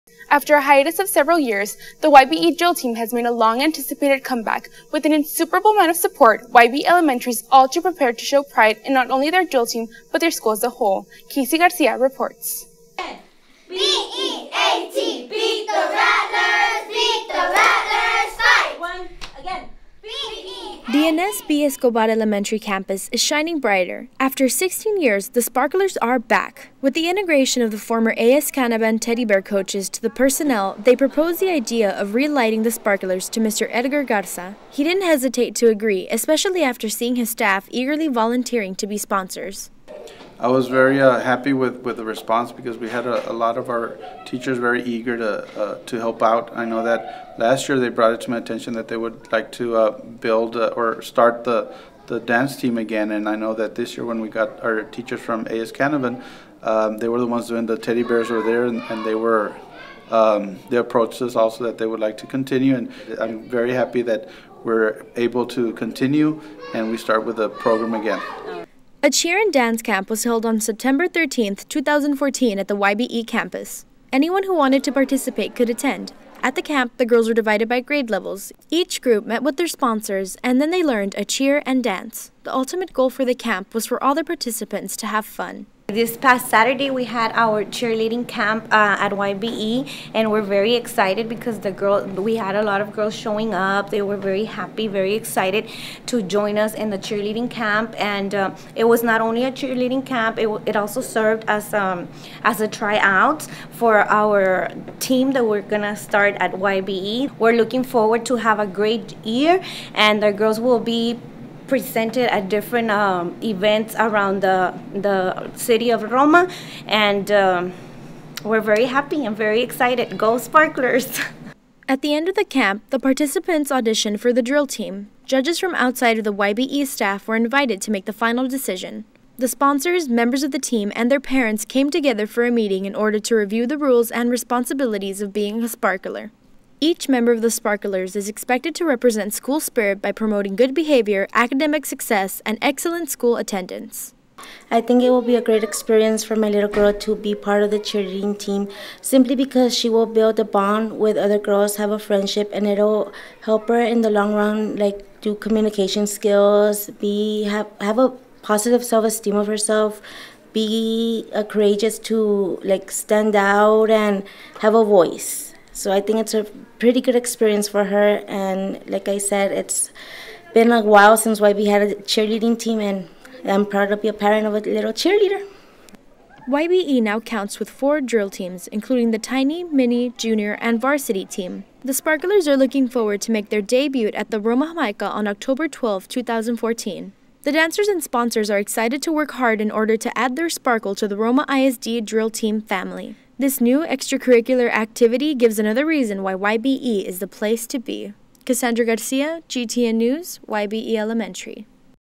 Y.B.E. Drill Team
Episode #416: After a hiatus of several years, the Y.B.E. Drill Team has made a long anticipated comeback. With an insuperable amount of support, Y.B.E. Elementary is all too prepared to show pride in not only their drill team but their school as a whole. Senior Video Journalist: Kassandra Garcia.